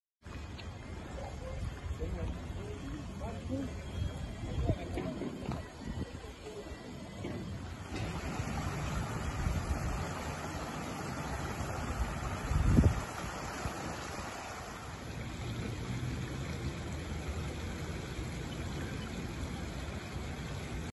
Así se observa el Río Alseseca con tonalidades rojizas debido a una supuesta descarga clandestina
Así se observa el Río Alseseca con tonalidades rojizas debido a una supuesta descarga clandestina de una fábrica en la zona de La Providencia.